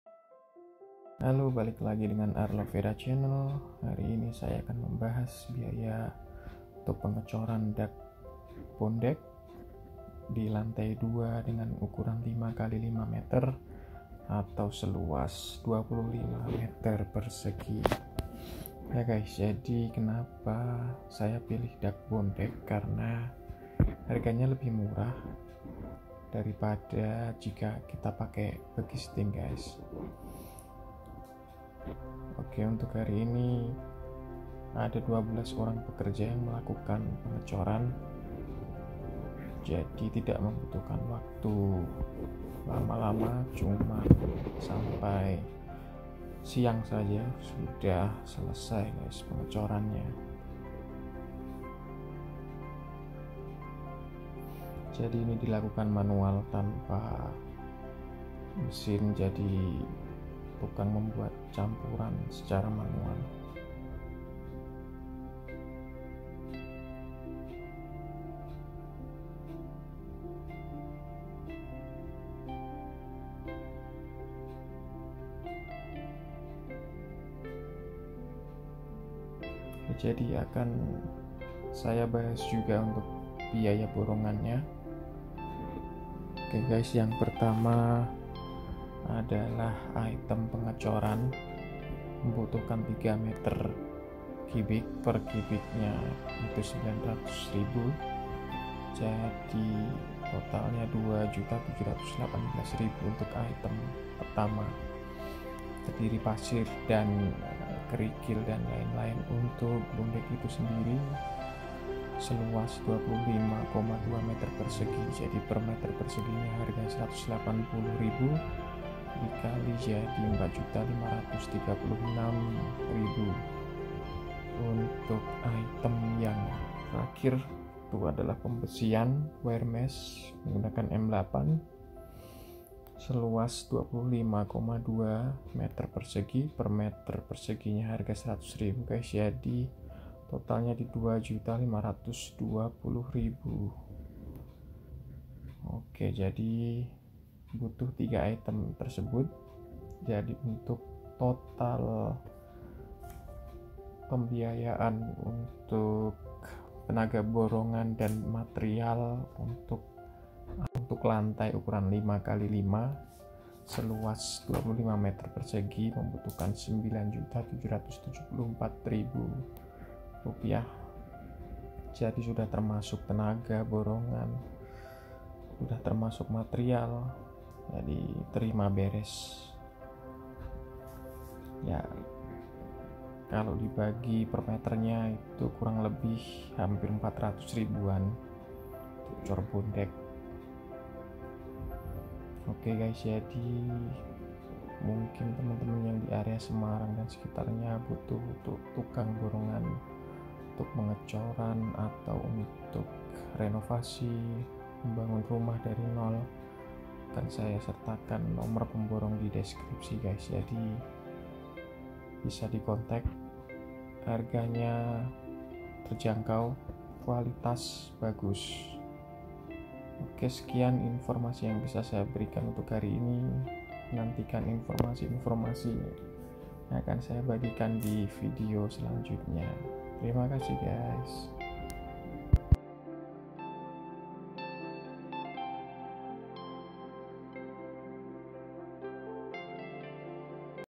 Murah‼️ Borongan Dak Bondek Rumah 2 Lantai
Berikut video pengecoran dak bondek ukuran 5x5 meter atau seluas 25 m2 harga lebih murah dibanding memakai bekisting.
Dikerjakan oleh 12 pekerja secara manual tanpa mesin.
Bagi yang membutuhkan jasa borongan area semarang dan sekitarnya bisa menghubungi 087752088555

Musician: @iksonmusic